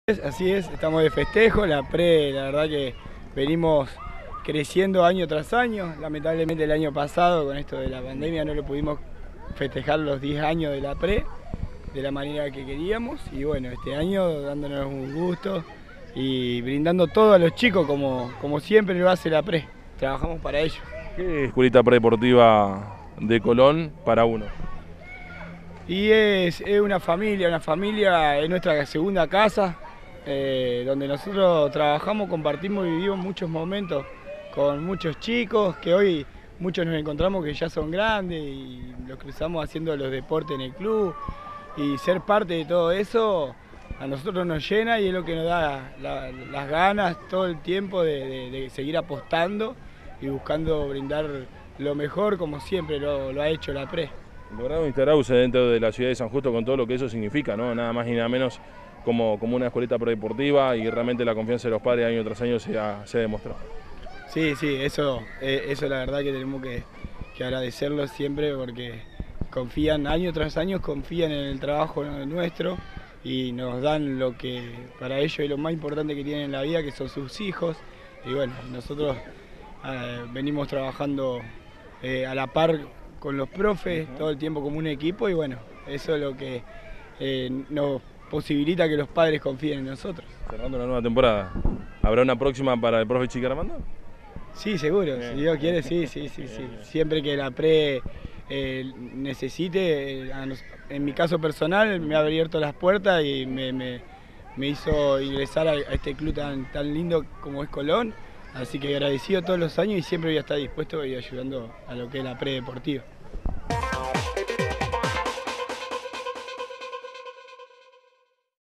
NORBERTO ARMANDO – Club Colón
Realizaron el festejo por los 11 años de vida de la Escuelita Pre Deportiva